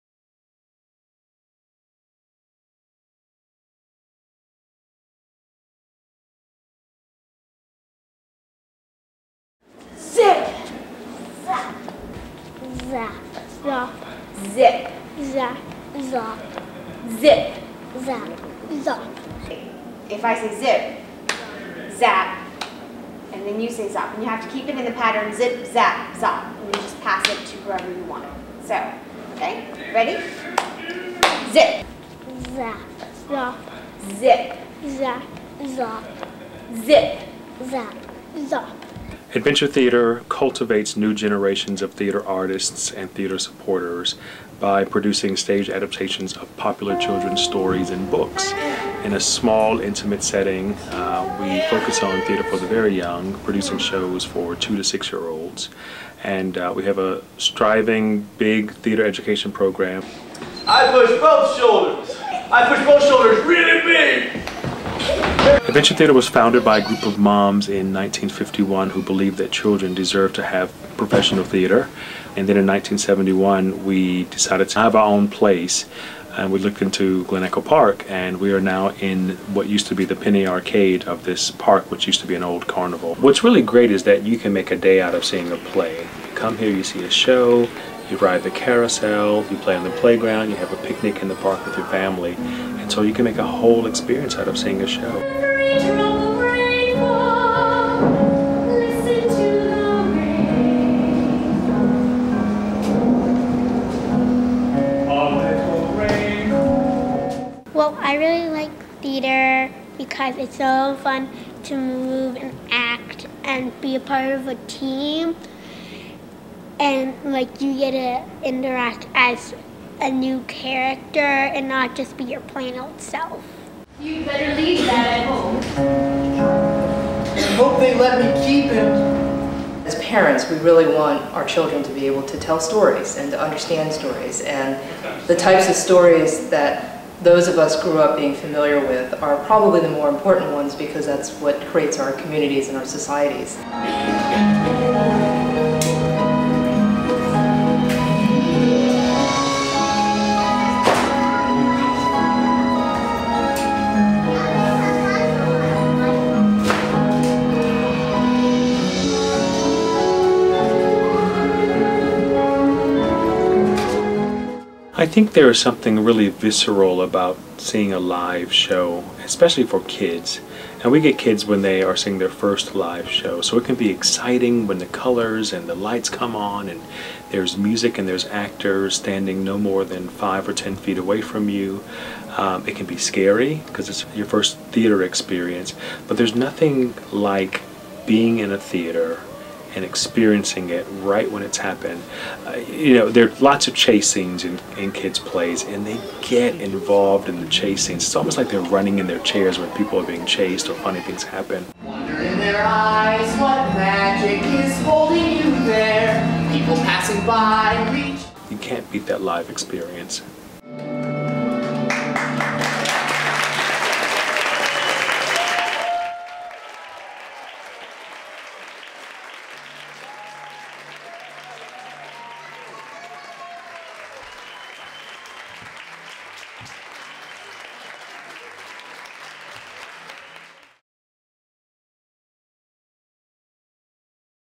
Adventure Theater
Produced as part of a documentary production class of Silver Spring, Maryland-based arts nonprofit Docs In Progress. By Intro to Documentary Production 2010 participants Melissa Grance, Henrick Gronhaug, and Peter Johnsen. Profile of Adventure Theater in Glen Echo Park, MD.